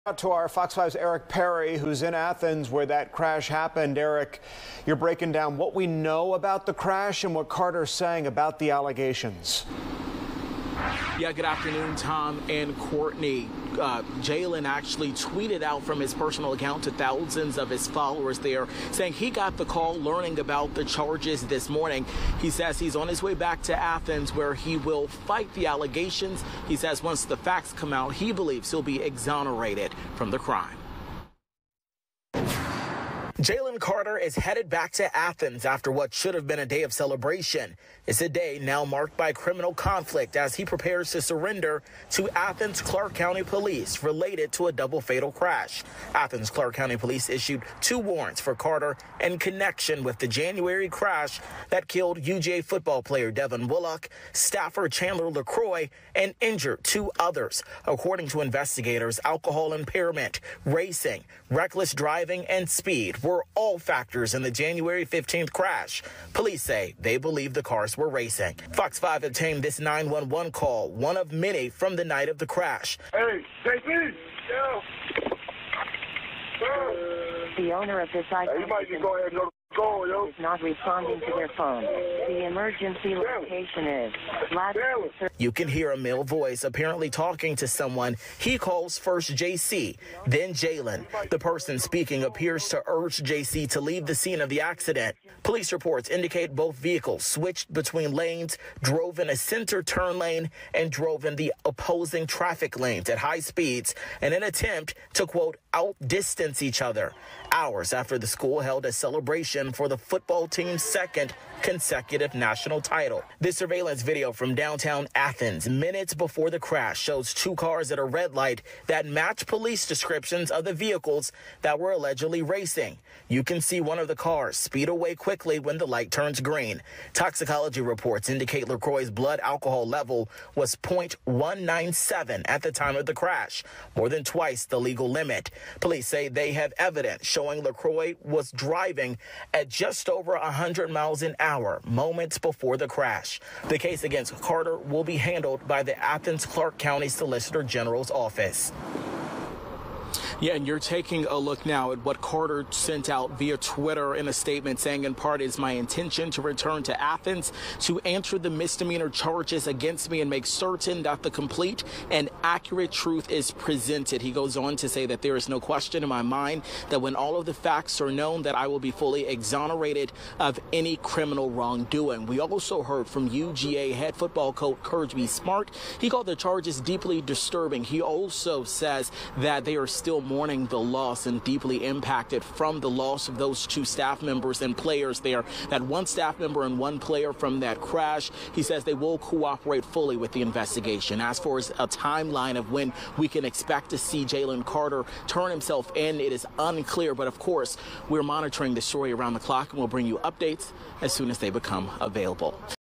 Surveillance video from Downtown Athens minutes before crash
Surveillance video from Downtown Athens minutes before the crash shows two cars at a red light that match police descriptions of the vehicles that were allegedly racing. You can see one of the cars speed away quickly when the light turns green.